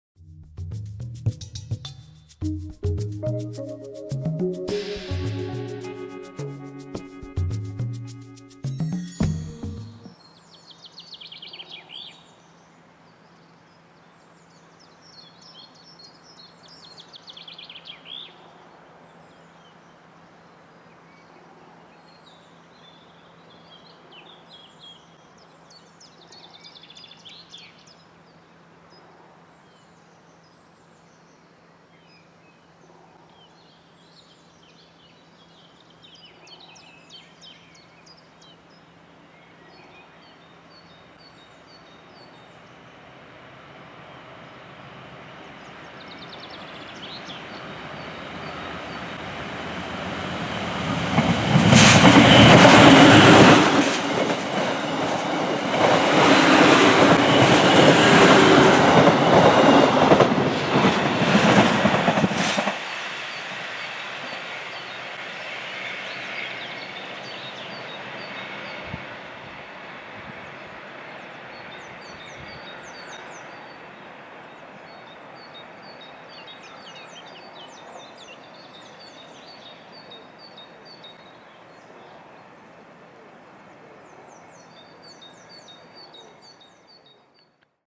3x BR 628 mit einem Sonderzug nach Bouzonville 2017
Aufgenommen am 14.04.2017 in Luisenthal (Saar)